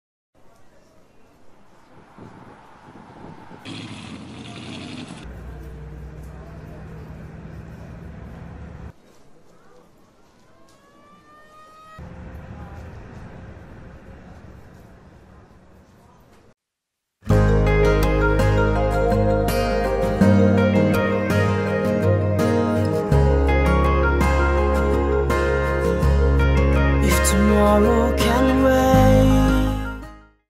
Tomorrow can wait / / Teaser
Hey Guys, Its been a while since I have uploaded.
The new video is gonna drop on 14th of February and here is a short teaser for it. I can't wait to show the video to you guys and I hope this teaser excites you as much as it excites me.

The list of people who worked on it are down below.

Director / / Video / / Editing / /

Starring / /

Music Production / /